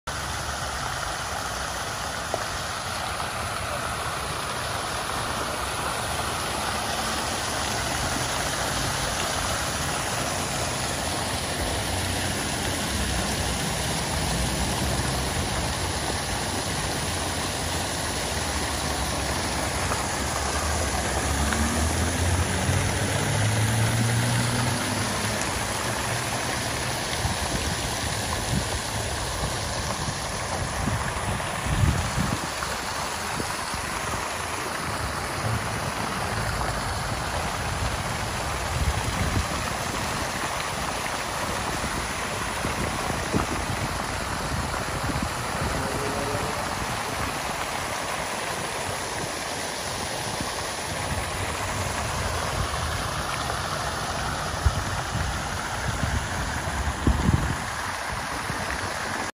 ATTENTION DOG LOVERS
This Fountain of Dogs is absolutely adorable! it’s located at the South Main Plaza in downtown Mount Vernon, Ohio. There are 18 cast aluminum dogs and Puppies, one cat, a few birds and one bone….lol it’s well worth the visit and will make you smile ear to ear.